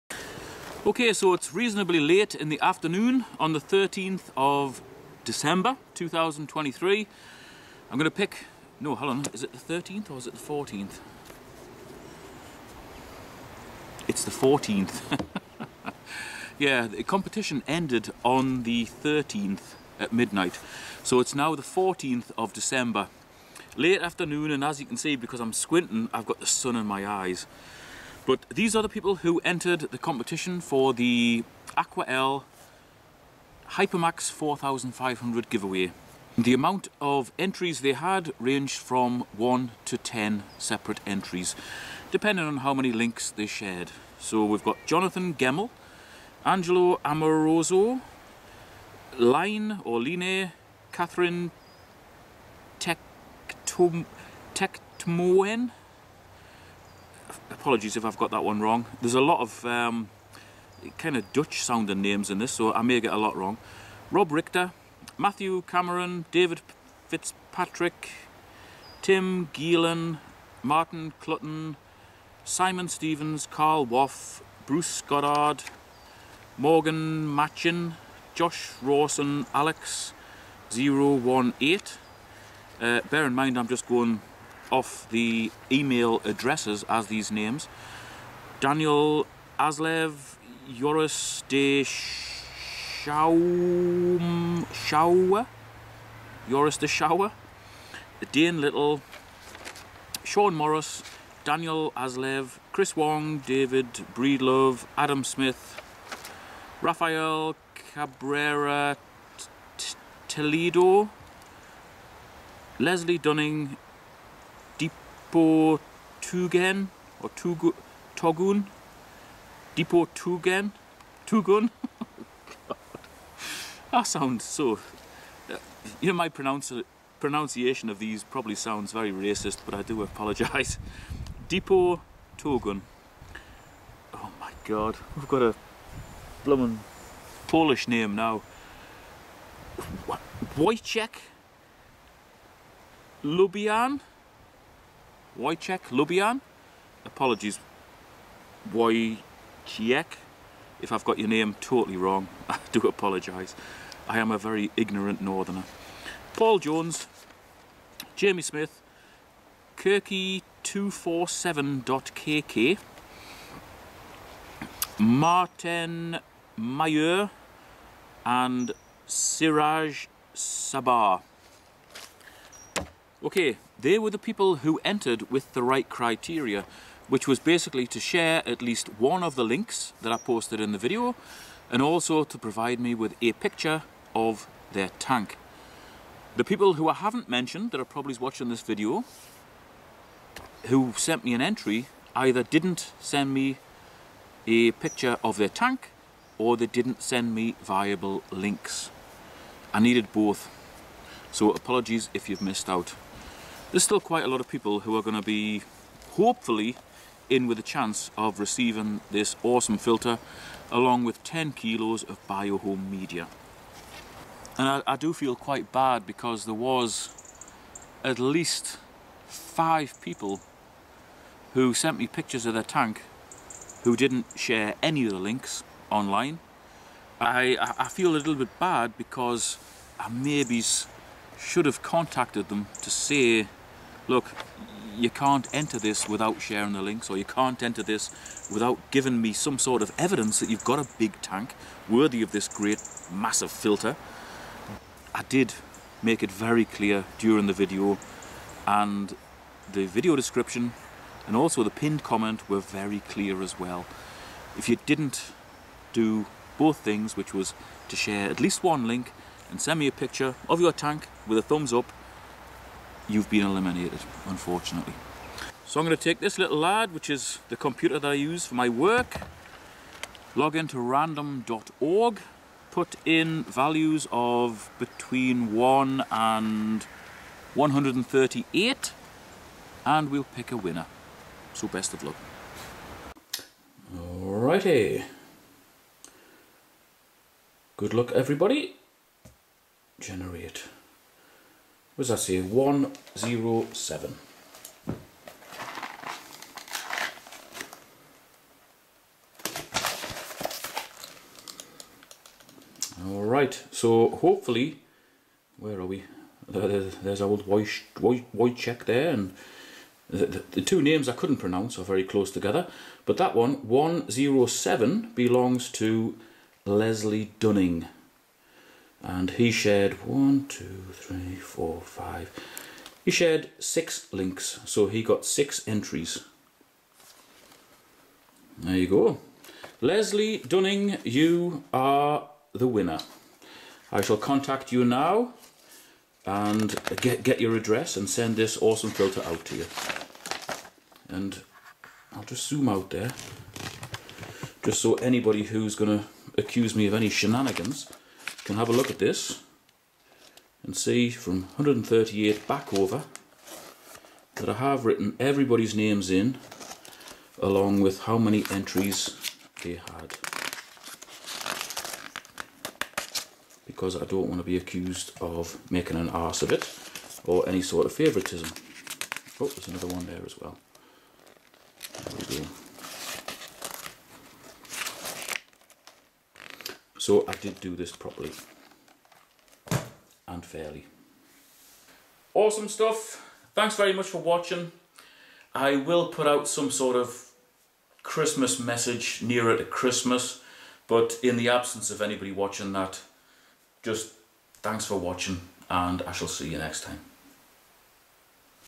Hypermax 4500 Filter Winner
Announcing the winner of the Hypermax 4500 canister filter.
Thanks to everyone took the time to enter and there were some beautiful pictures of tanks sent as part of the entry requirements which I really enjoyed looking at.
I will be giving some more aquarium related gear away on my 'PondGuru' channel after Christmas as well as outdoor gear on my 'Thousand Yard Stare' channel.
I'd intended to do all the give aways before Christmas but due to time restraints that hasn't happened unfortunately.